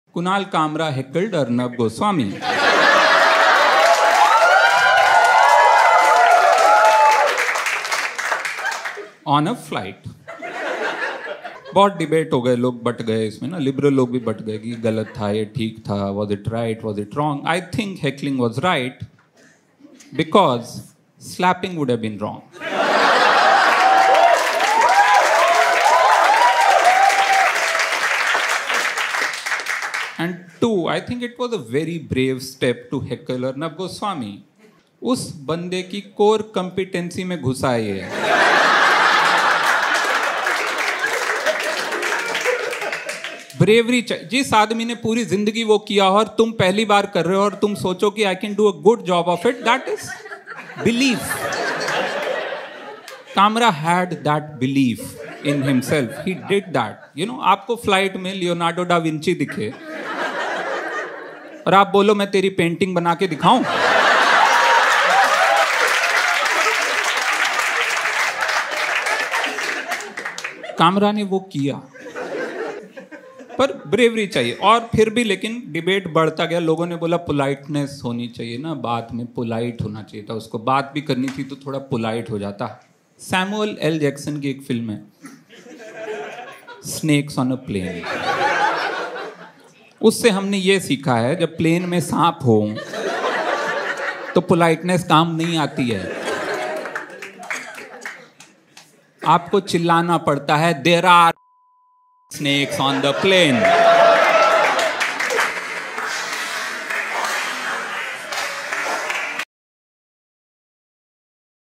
Varun grover's view on Kunal Kamra's heckling Arnab Goswami
I just added few images in images to relate Varun's jokes to layman after these many years :)